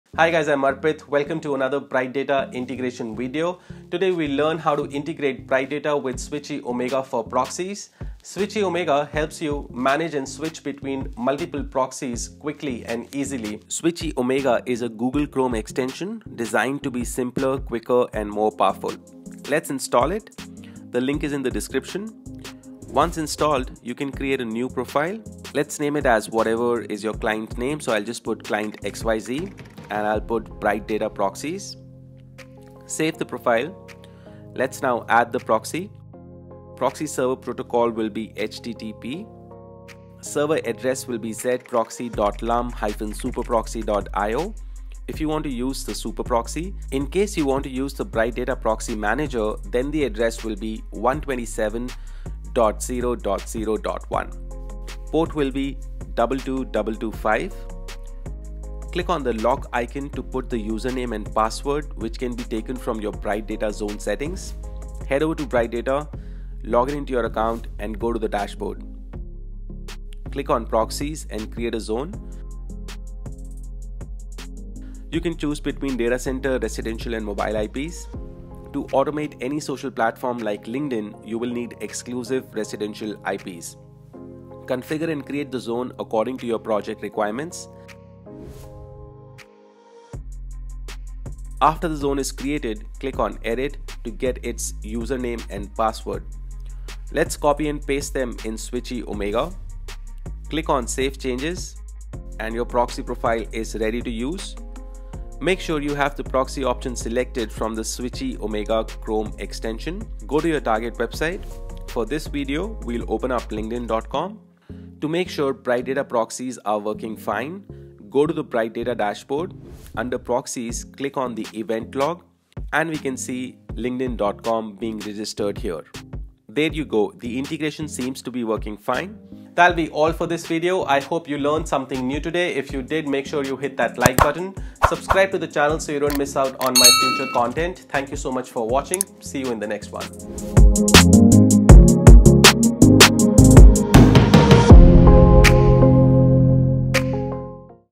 SwitchyOmega Google Chrome Proxy Setup! Best Chrome Extension To Manage Proxies.
Today we’ll learn how to set-up and manage proxies for Google Chrome.

SwitchyOmega is a free Google Chrome extension designed to be simpler, quicker & more powerful.

SwitchyOmega helps manage and switch between multiple proxies quickly & easily.

We’ll learn how to integrate Bright Data proxies with SwitchyOmega so you can perform your job seamlessly.